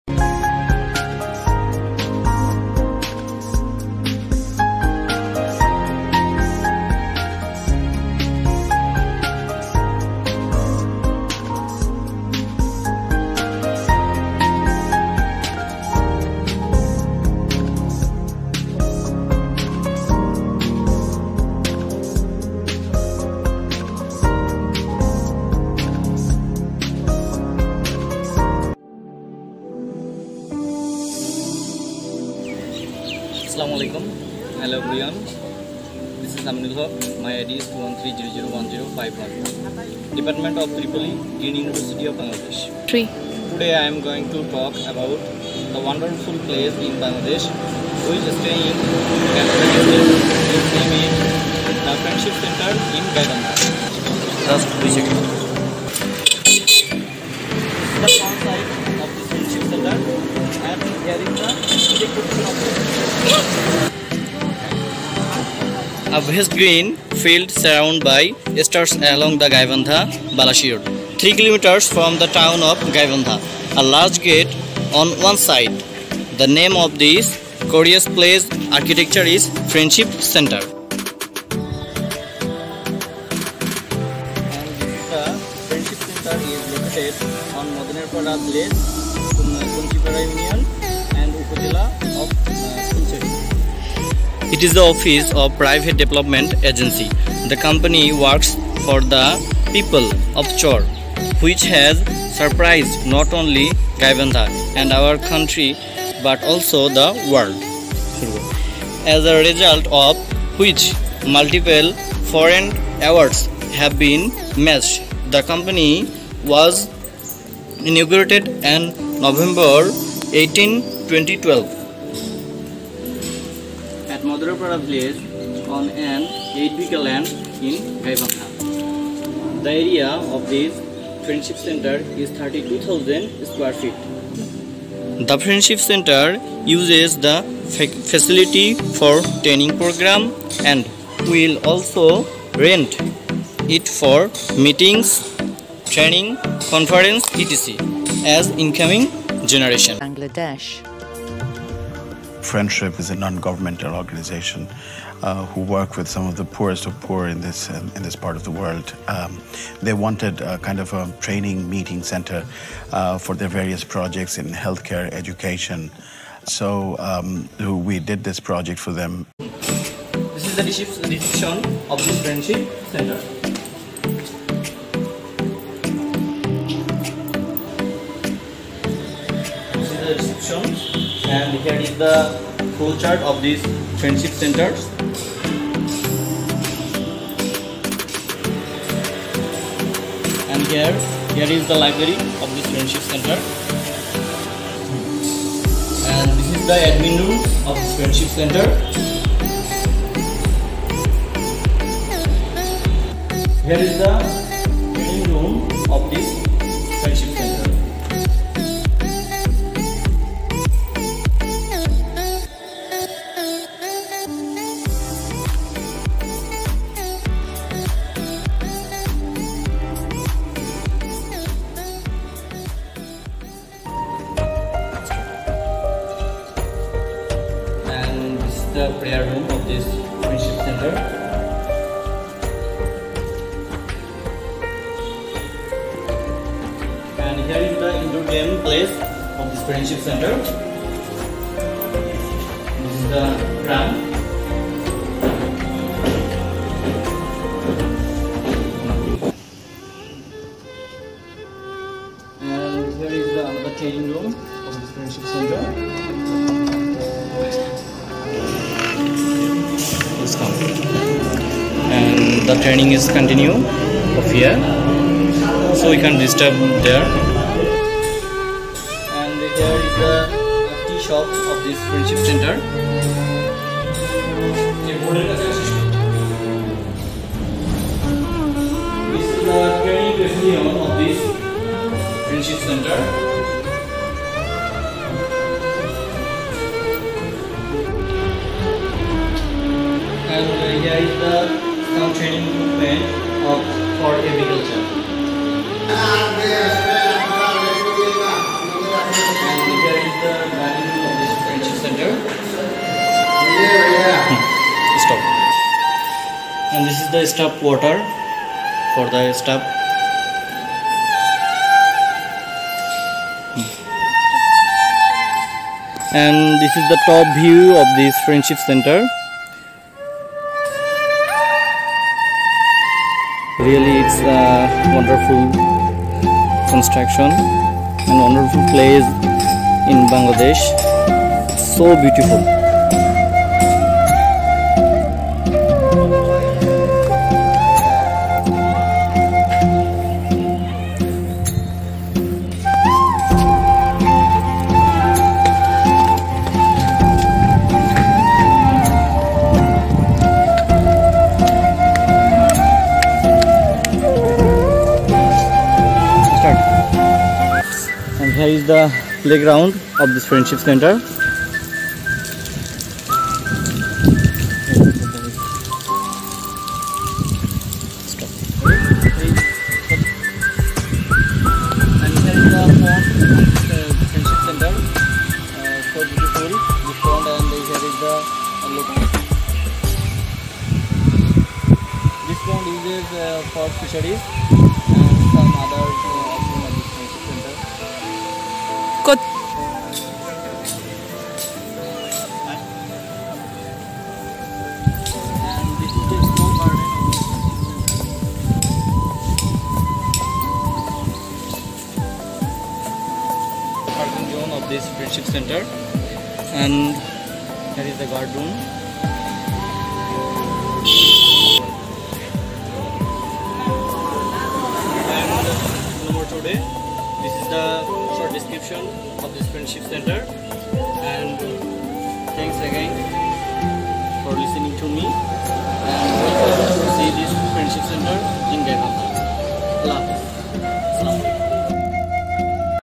A tour to Friendship Center, Gaibandha, Bangladesh
EAP Course Project Video by Md  Amenul Haque, Undergraduate student, Department of EEE, Green University of Bangladesh Course Tutor: Md. Didarul Islam, Lecturer in English, Green University Language Center